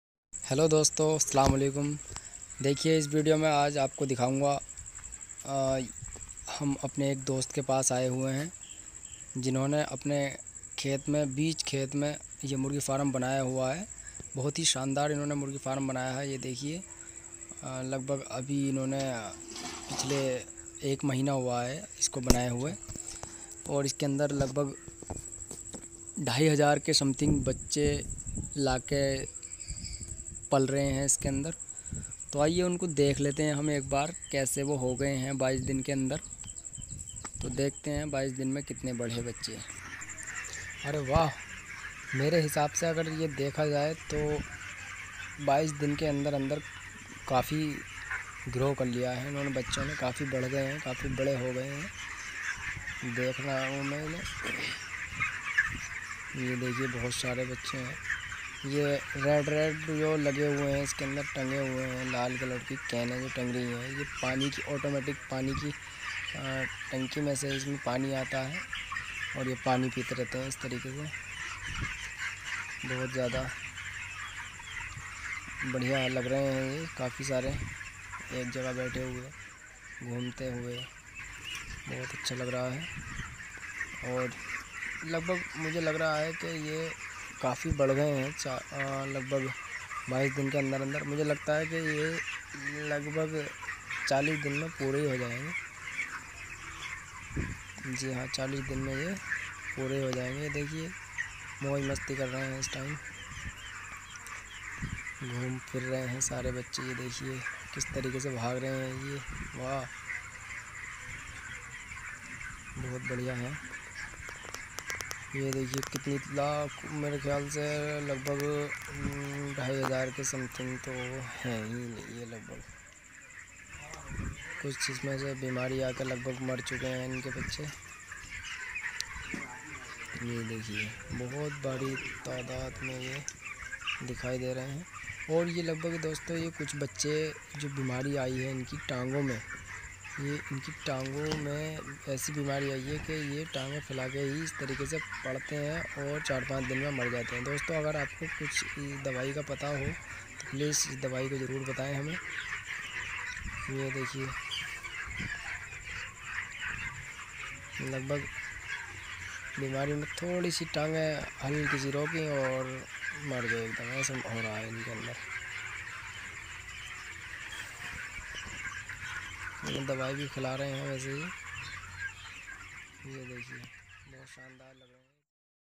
Poultry Farm (Chicken Farm) in village field Uttar Pradesh
Poultry farming in my village
chicken farm village in uttar pradesh.
murgi farm kaise hota hai ek baar video jaror dekhe.